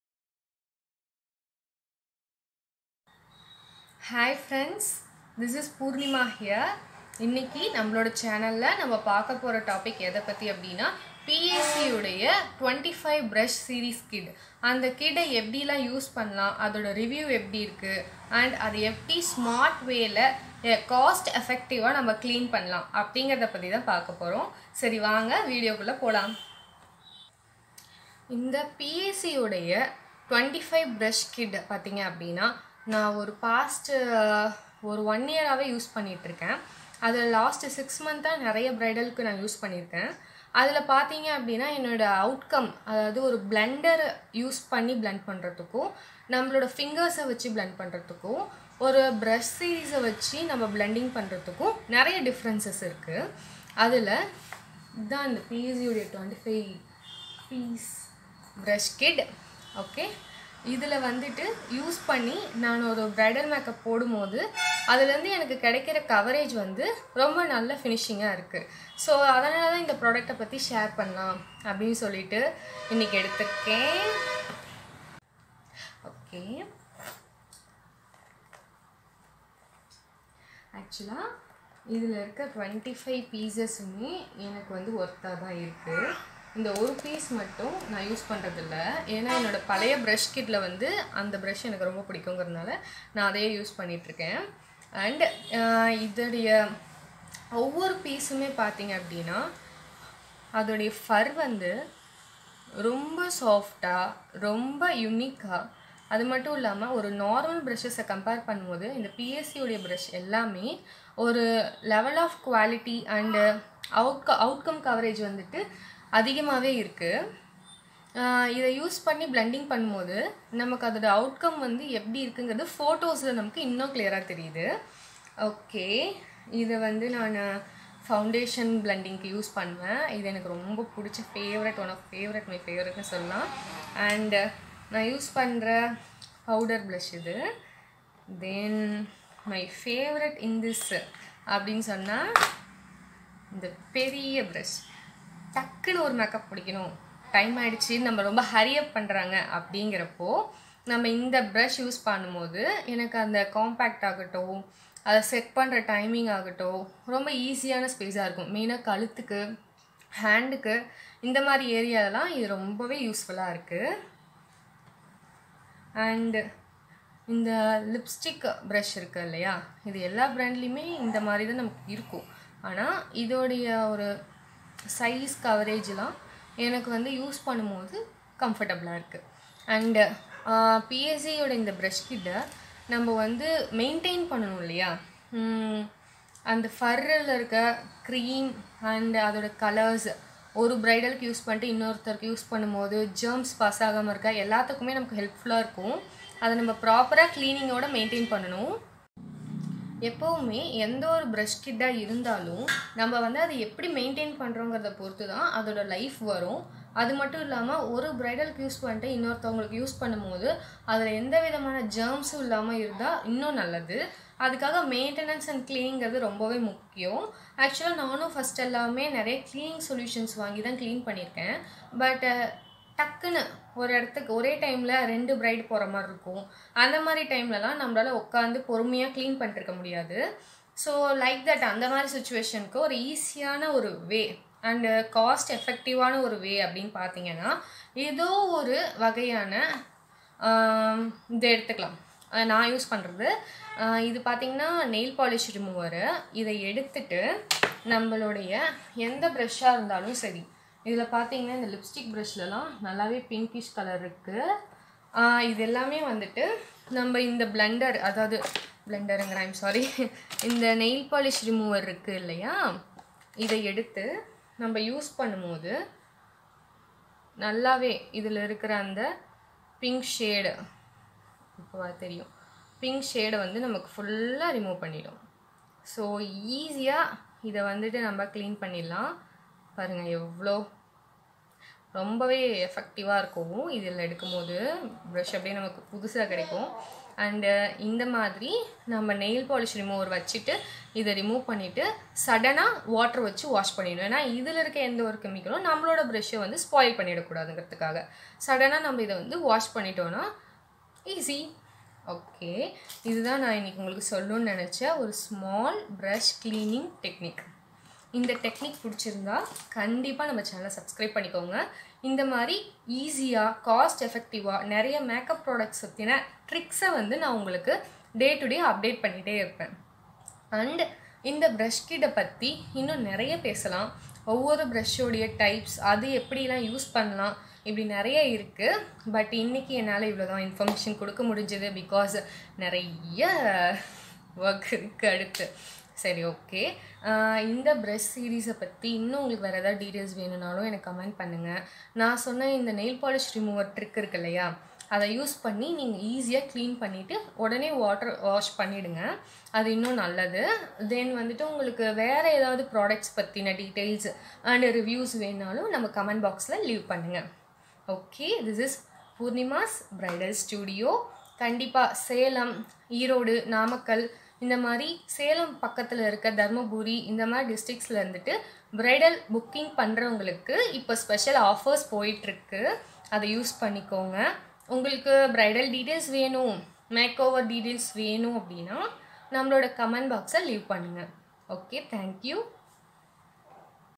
PAC_brush_review(not sponsored)||Tamil||professional makeup artist
This vedio is about a brush review
I am a pro makeup artist here to explain about products and maintenance of kid in easy way

🥳For makeup booking